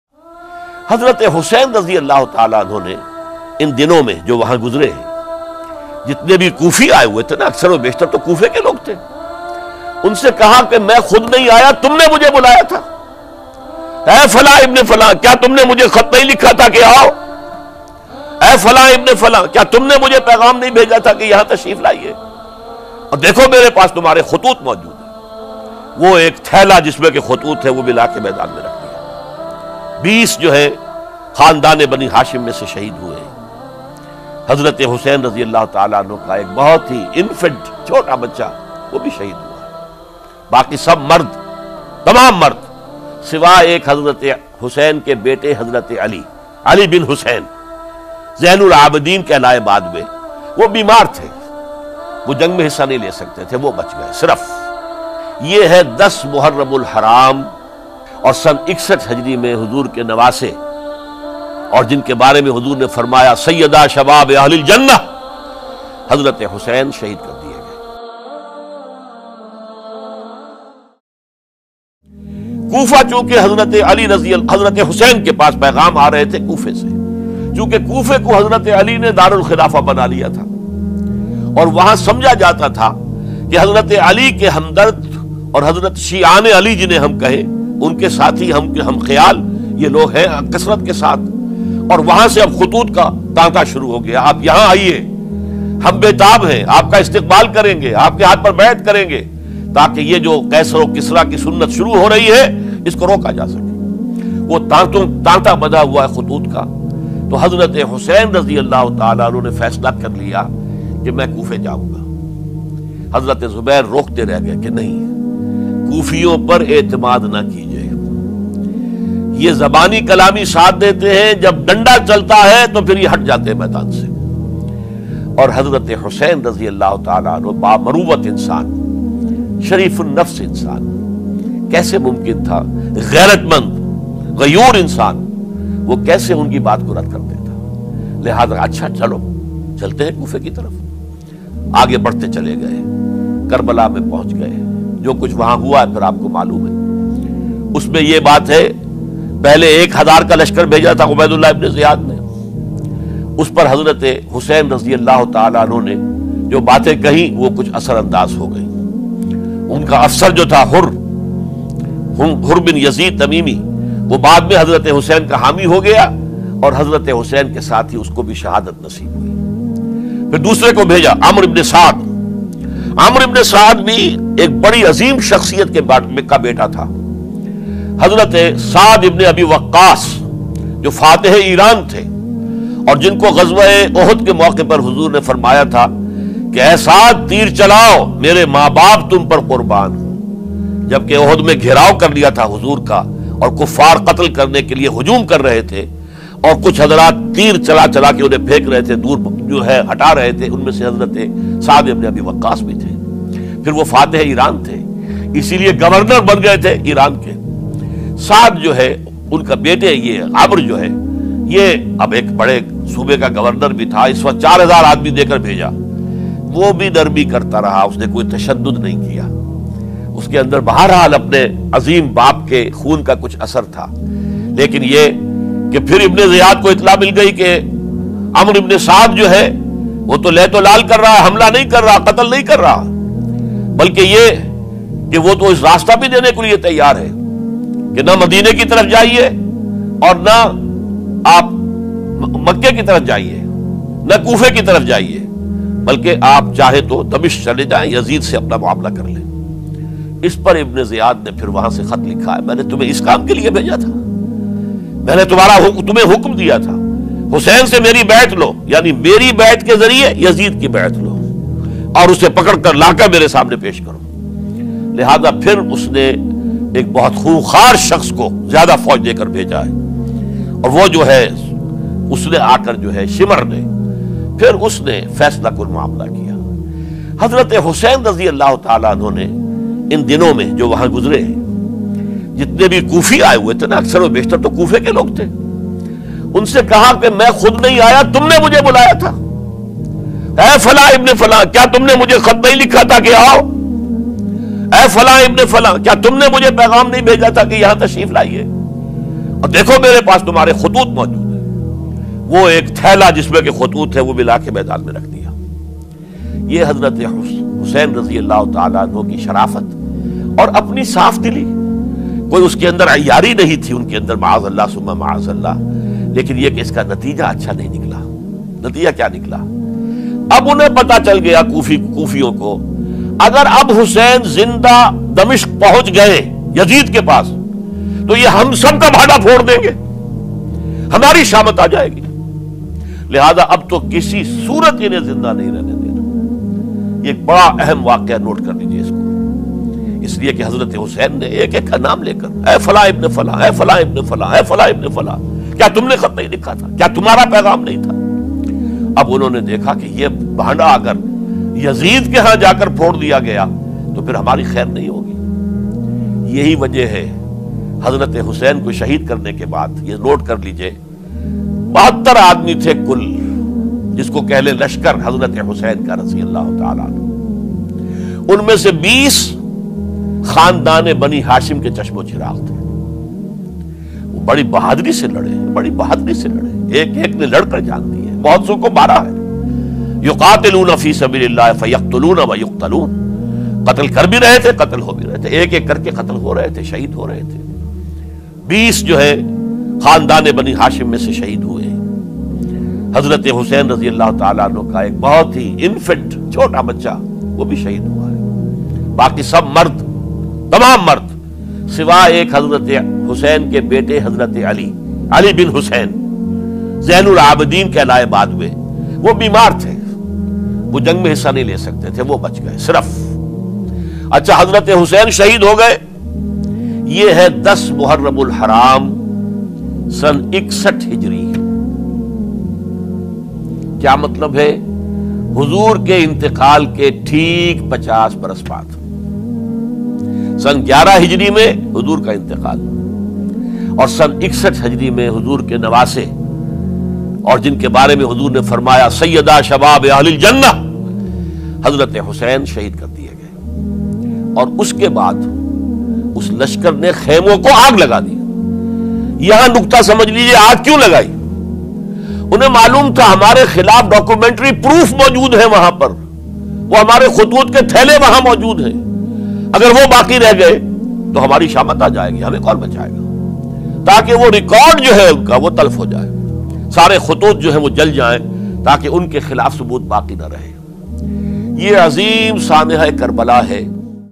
Imam Hussain R.A Ki Shahadat Kaisay Hoi? - Rula Dene Wala Bayan - Karbala Ka Waqia - Dr Israr Ahmed
عبیداللہ بن زیاد  ،  شمر اور عمر بن سعد کون تھے؟